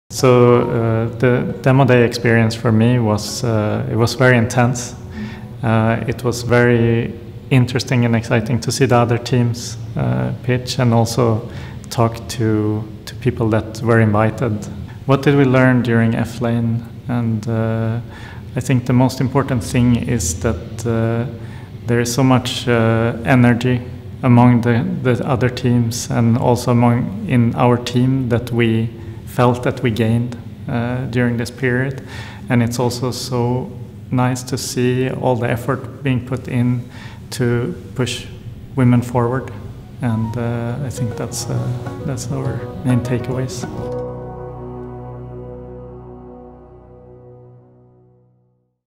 Interview with BOOST Thyroid at Demo Day 2019
Interview with last year's F-LANE Accelerator participant from the start-up BOOST Thyroid.